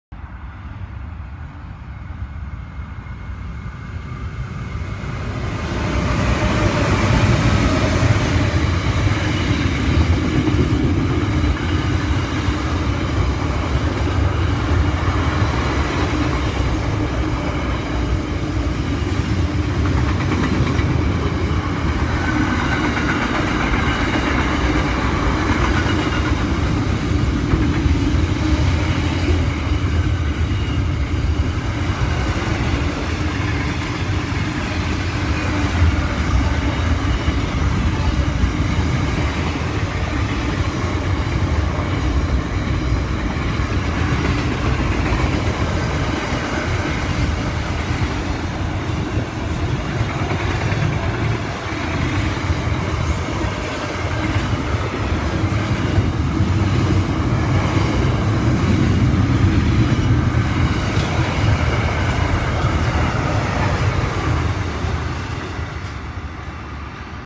NR48-NR81 Gheringhap Loop
On this day dad and I woke early.  Dad and I went for a drive and saw that the Gheringhap Loop had a green light to allow passage of a train to proceed towards Melbourne (Up).  We waited for a short time, and then this train, with Pacific National locomotives NR48-NR81 came rushing through with 74 wagons in tow.  The trains journey originated in Adelaide, South Australia.
Some of the NR class locomotives have names.  NR48 is named Northam.  NR81 has no name.

Trainspotting, Lower Victoria.
2019